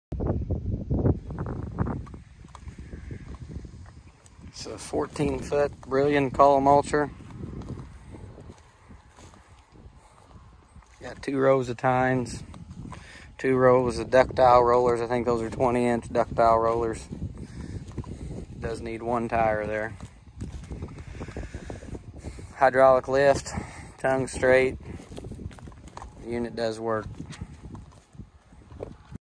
BRILLION H164 For Sale
Brillion Culti-mulcher, ductile rollers, nice older mulcher, Danish tine, hydraulic lift, will need one tire, unit rolls great, was used the fall.